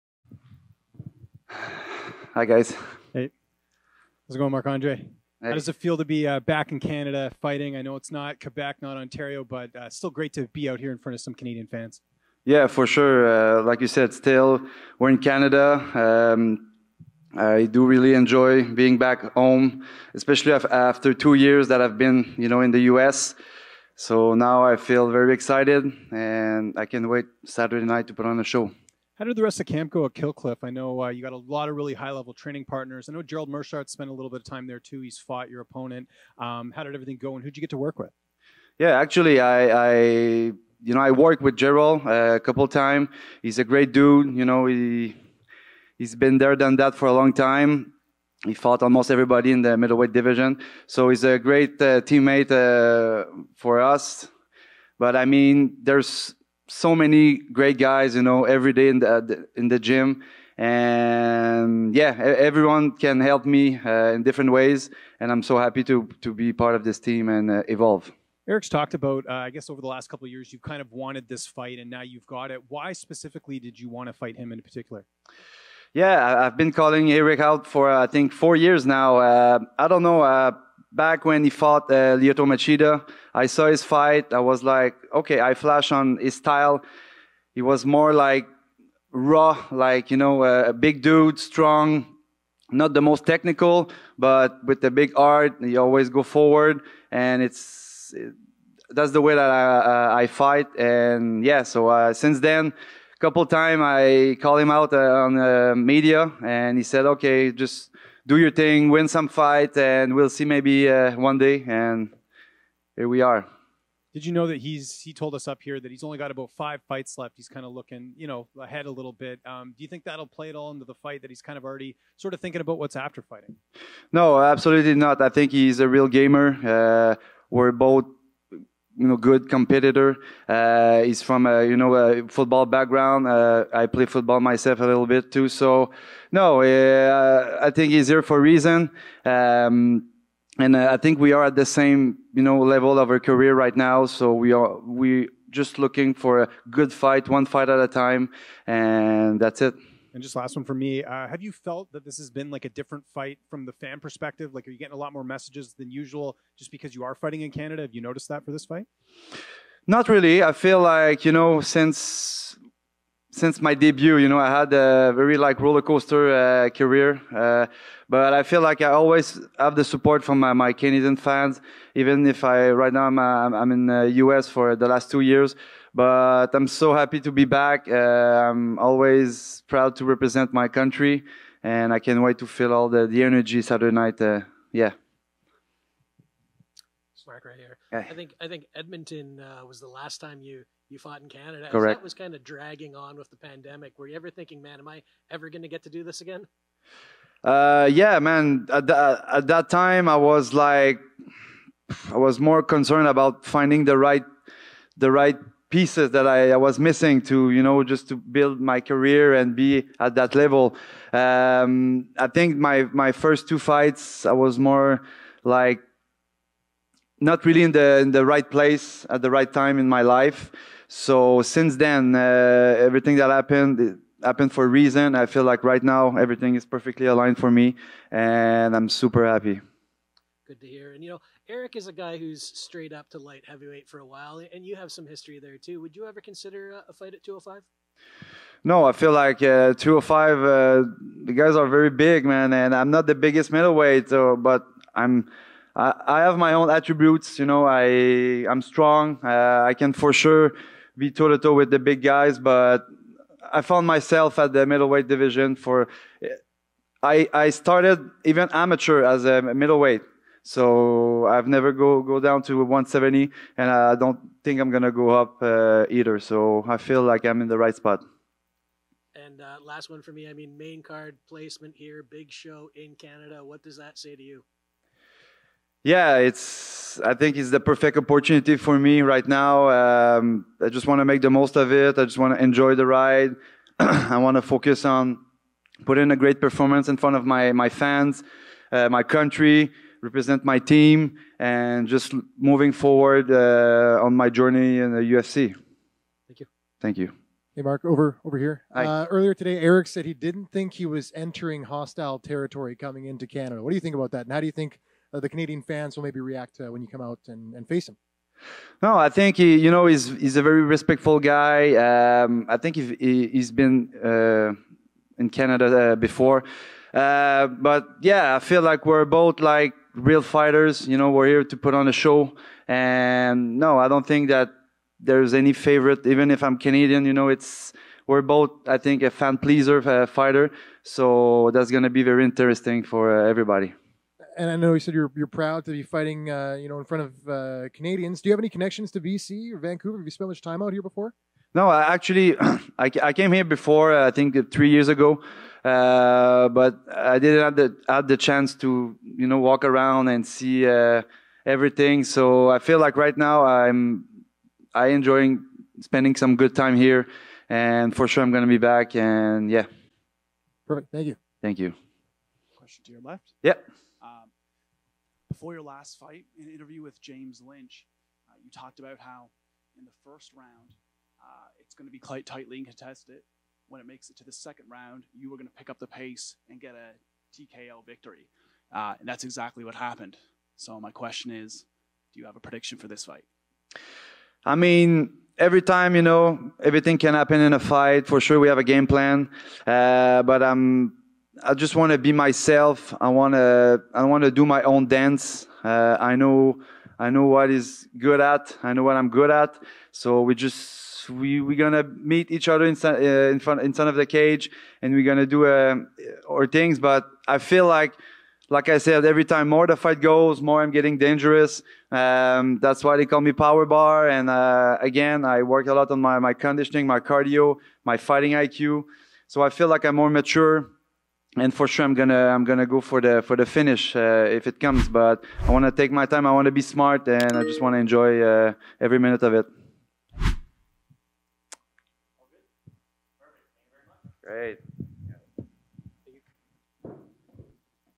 Marc-Andre Barriault | UFC 289 Media Day Interview
Marc-Andre Barriault speaks with the media ahead of his fight at UFC 289